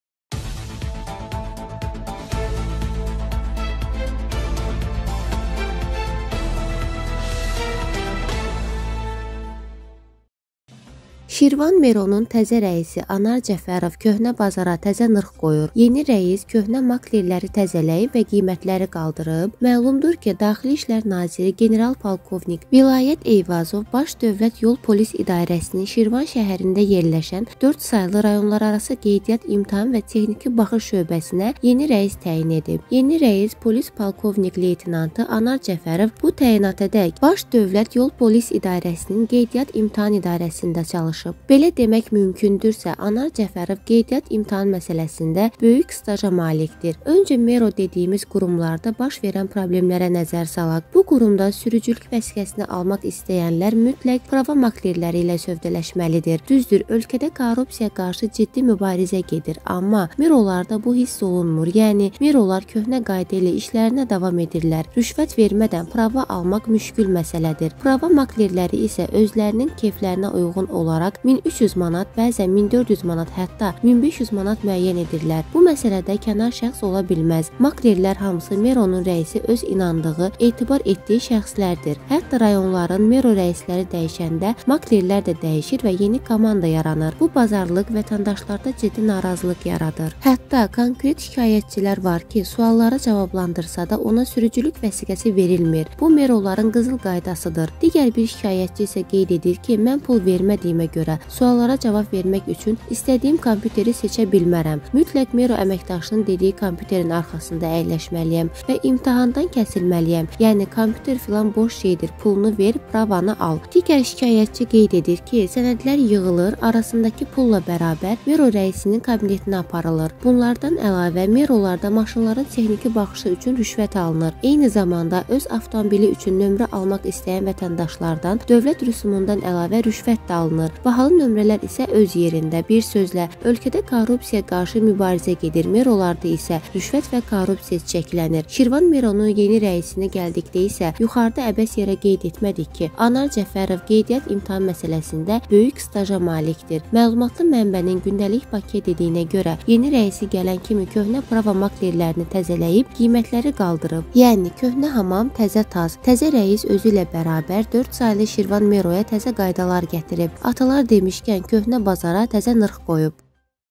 Şirvan “MERO”-da Qiymətlər “ceyran belinə” qalxdı...
Şirvan MERO-nun təzə rəisi Anar Cəfərov “köhnə bazara təzə NIRX” qoyur…- Yeni rəis “köhnə makler”ləri “təzələyib” və qiymətləri qaldırıb?-